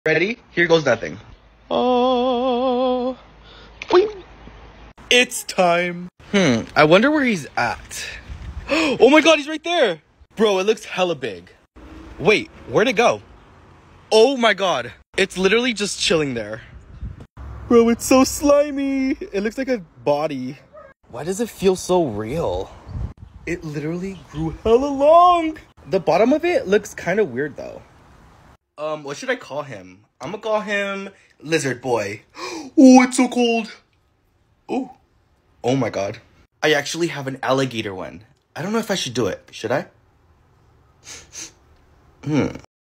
Throwing Water Growing Lizard in Pool 🦎💦 (PART 2)
I hope you like the results on the growing lizard!
Should I throw in the Alligator next? 🐊😱